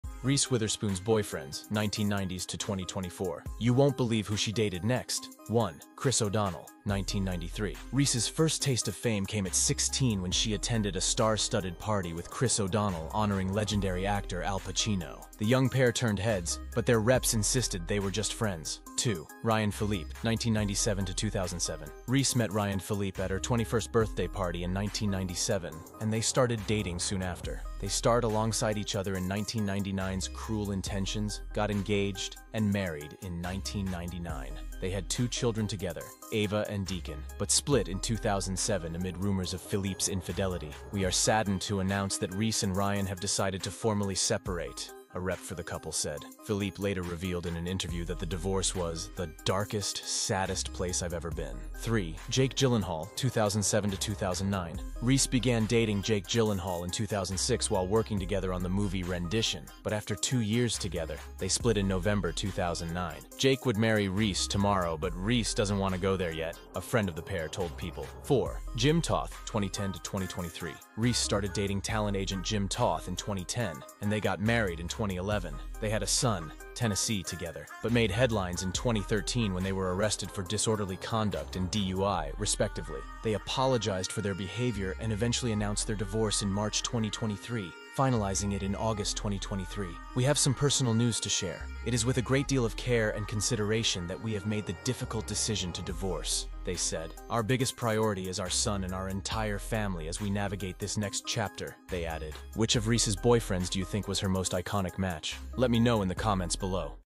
Reese Witherspoon's Boyfriends (1990 - 2024) | Infotainment by Hamza
Get ready for a walk down memory lane! In this video, we're counting down Reese Witherspoon's most iconic boyfriends from the 1990s to 2024. From her early days as a teen star to her current status as a Hollywood A-lister, Reese has had her fair share of high-profile romances. Who was her most iconic match?